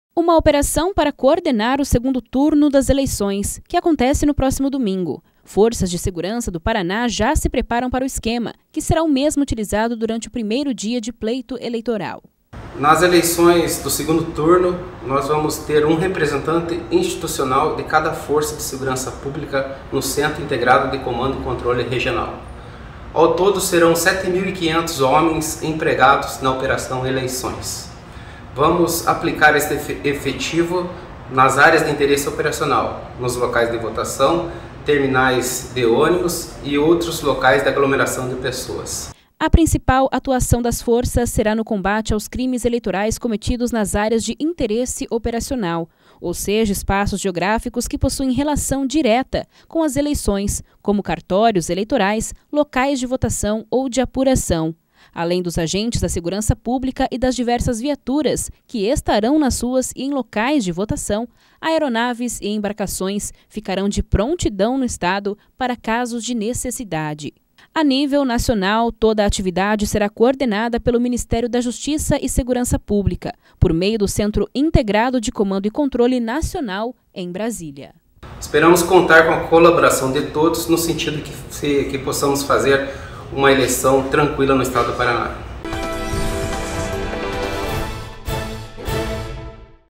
Forças de segurança se preparam para operação eleições que será realizada no próximo domingo
A nível nacional, toda a atividade será coordenada pelo Ministério da Justiça e Segurança Pública, por meio do Centro Integrado de Comando e Controle Nacional, em Brasília.